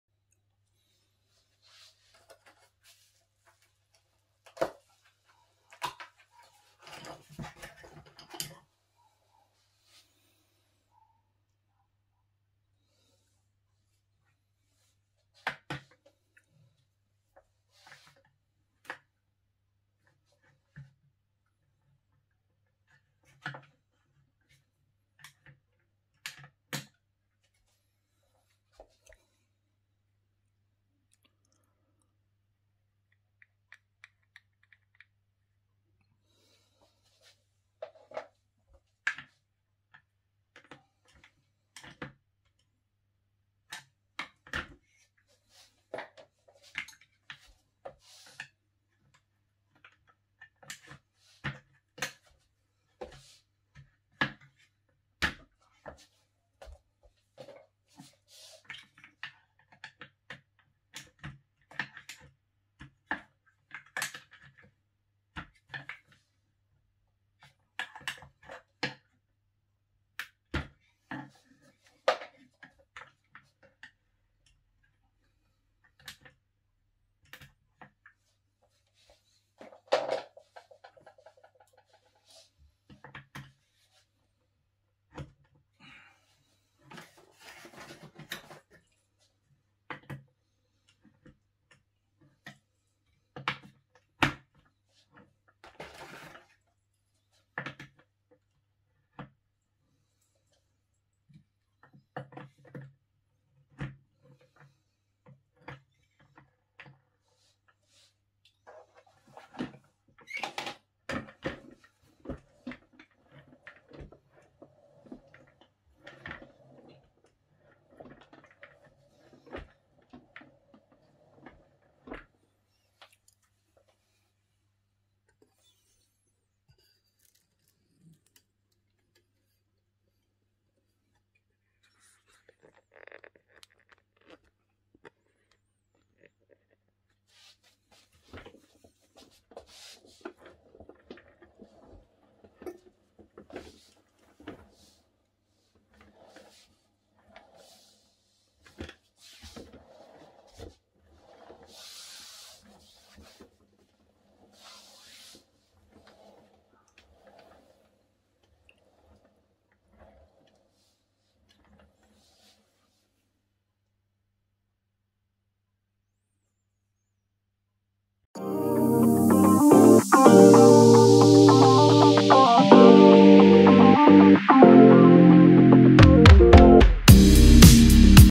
Installing Office Chair Wheels with Rollerblade Style Wheels Replacement
BUY Office Chair Wheels with Rollerblade Style Wheels Replacement
Amazon Link US

Amazon Link UK